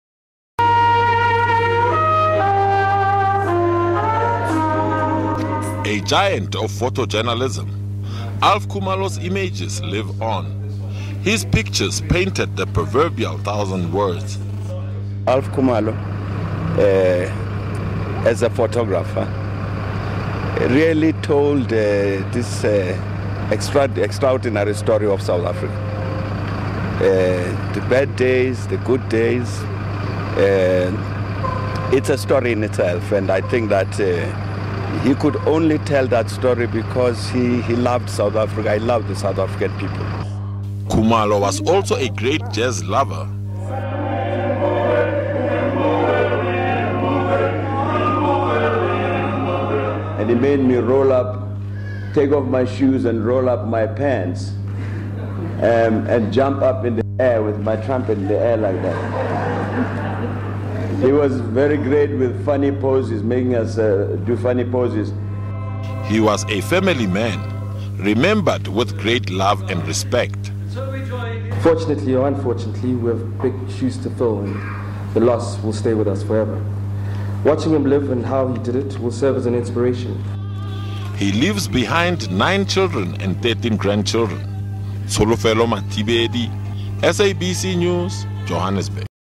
Alf Kumalo's son will miss his father dearly
Veteran photo journalist Alf Kumalo has been laid to rest.
Mourners gathered at the St Michaels church in Johannesburg to celebrate his illustrious life.